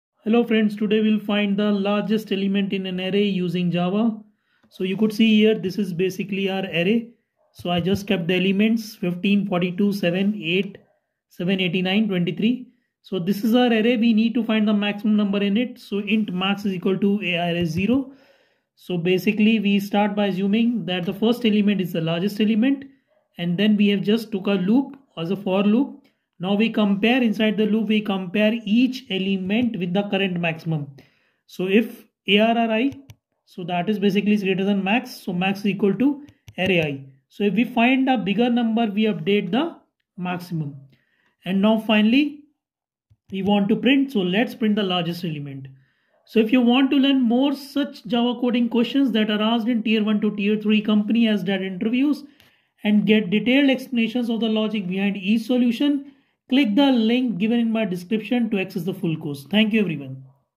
Java Program To Find Largest Element
Learn how to Reverse an Array in Java using the two-pointer method.

Perfect for interviews and SDET preparation.

Full Java course with more interview problems